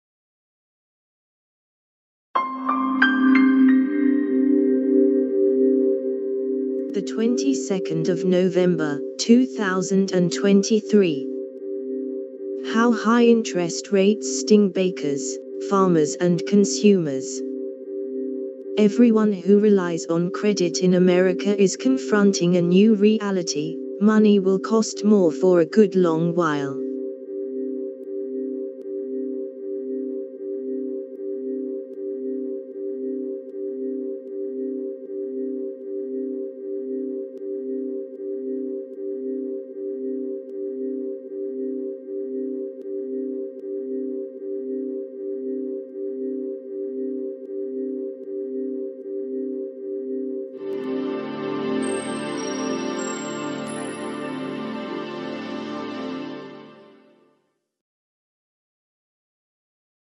[Latest]How High Interest Rates Sting Bakers, Farmers and Consumers
[Contents]
Everyone who relies on credit in America is confronting a new reality: Money will cost more for a good long while.